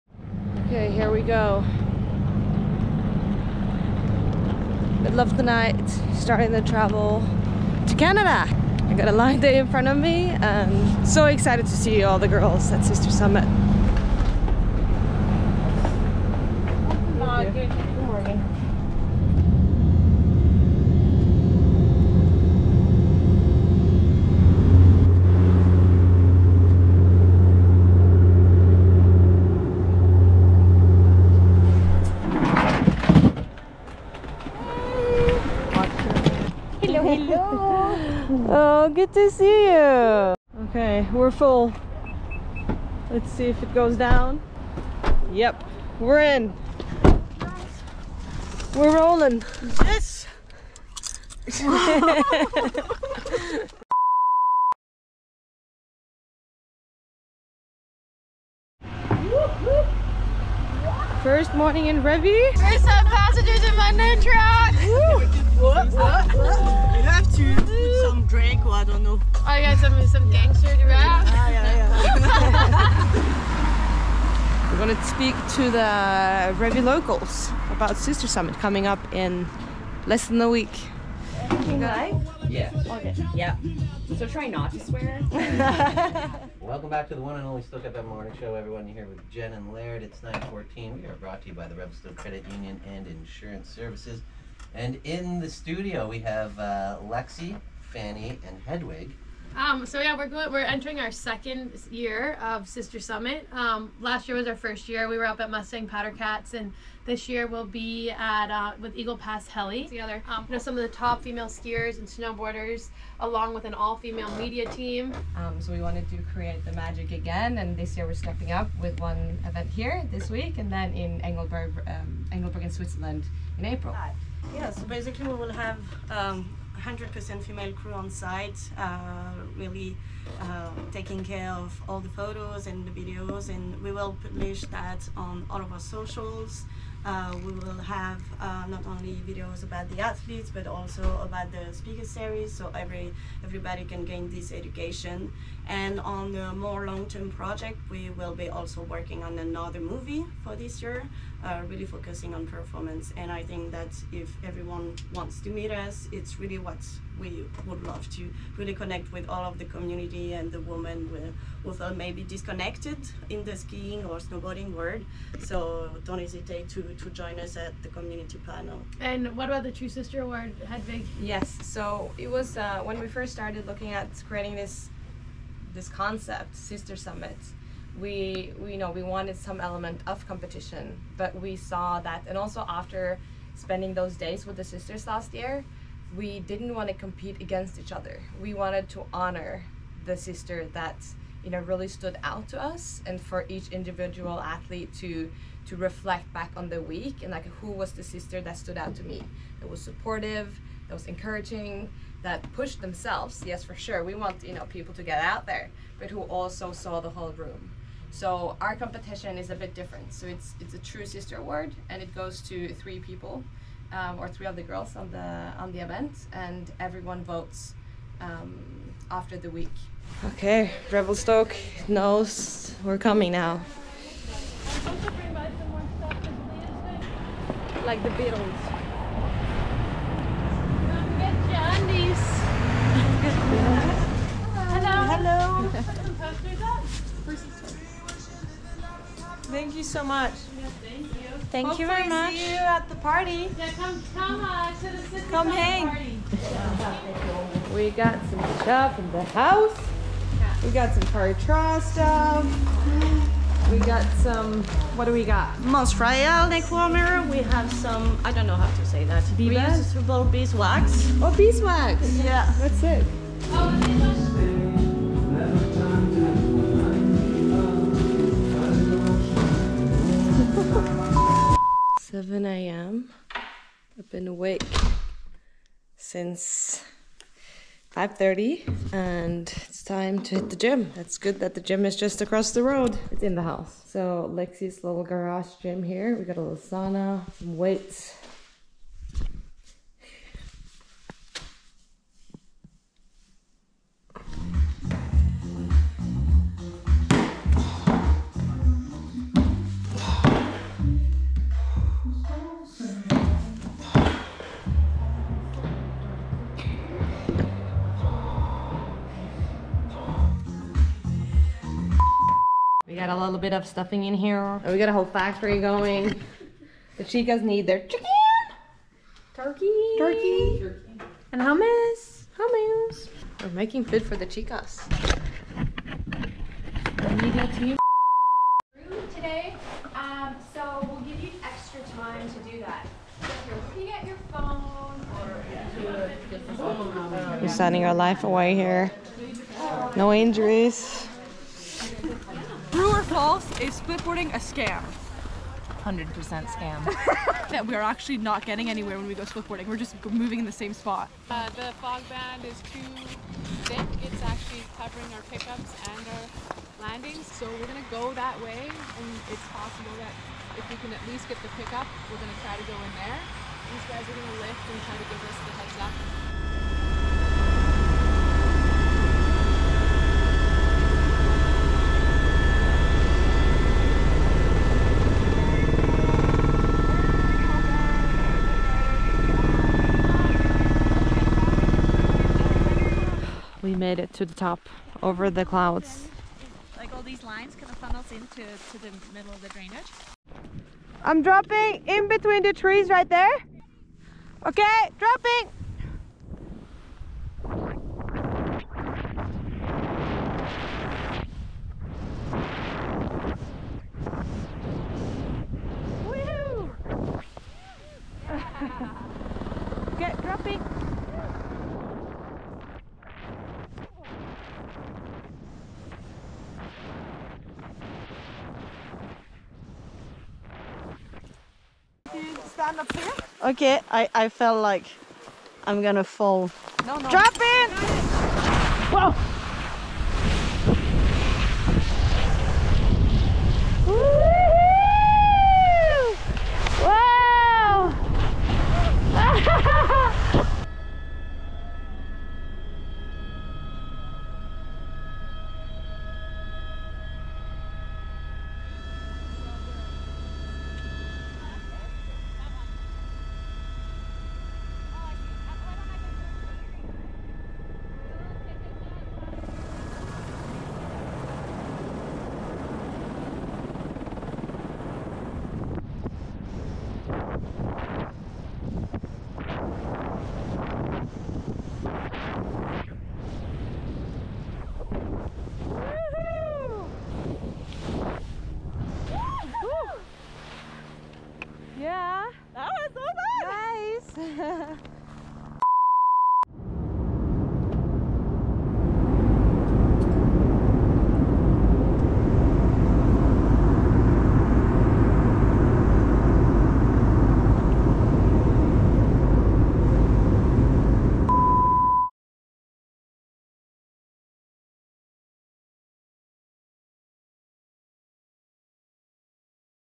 Hedvig UNFILTERED // Behind The Scenes from Sister Summit // Episode 2
Episode two of Hedvig Unfiltered, we’ll show you the raw, unpolished version of Hedvig. This time, Hedvig is in Canada to prepare and host the second edition of Sister Summit. Hedvig founded Sister Summit in 2022 with Lexi doPont and Fanny Avril, and now they are back! With more than 40 women together for skiing, learning and growing.

Edited by: Borja Azurmendi